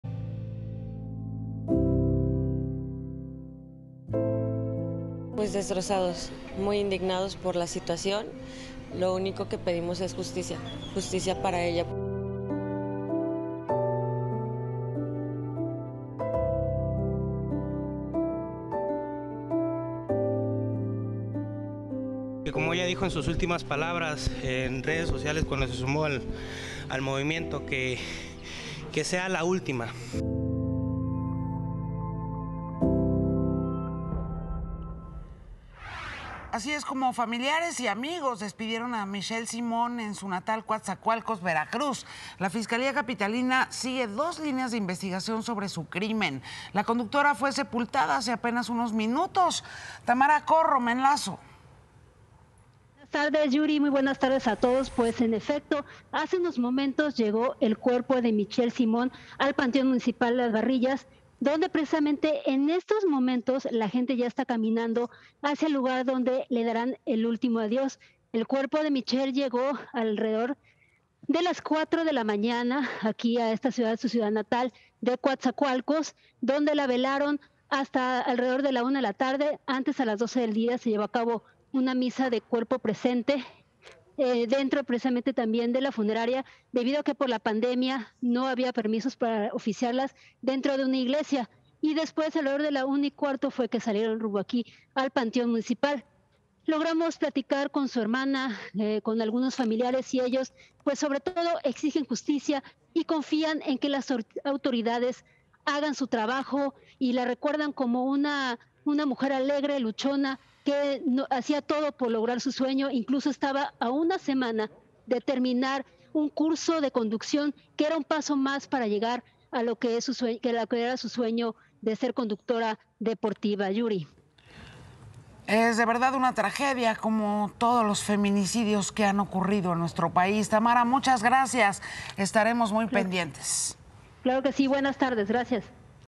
Velan y sepultan a Michell Simón en Coatzacoalcos, Veracruz | Noticias con Yuriria Sierra
Familiares y amigos despidieron a MichellSimón en su natal Coatzacoalcos, Veracruz. La Fiscalía de la CDMX sigue dos líneas de investigación sobre su crimen.